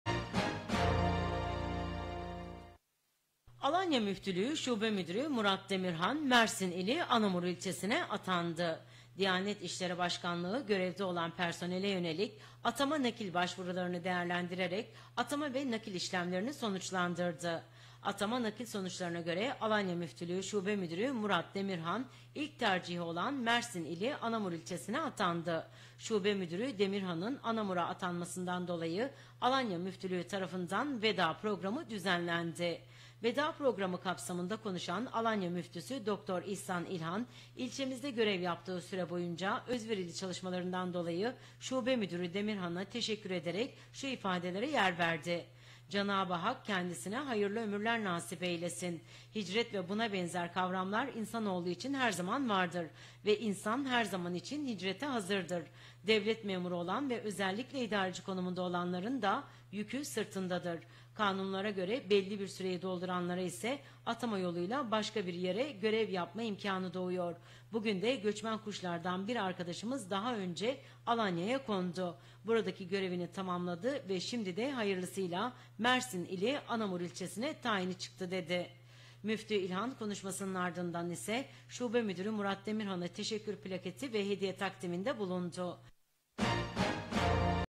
Alanya Şube Müdürü Murat DEMİRHAN Anamur'a Atandı
Alanya Müftülüğü Şube Müdürü Murat DEMİRHAN, Mersin İli Anamur İlçesine atandı.

Diyanet İşleri Başkanlığı görevde olan personele yönelik "Atama Nakil" başvurularını değerlendirerek, atama ve nakil işlemlerini sonuçlandırdı.

Atama nakil sonuçlarına göre Alanya Müftülüğü Şube Müdürü Murat DEMİRHAN ilk tercihi olan Mersin İli Anamur İlçesine atandı.

Şube Müdürü DEMİRHAN’ın Anamur’a atanmasından dolayı Alanya Müftülüğümüz tarafından veda programı düzenlendi.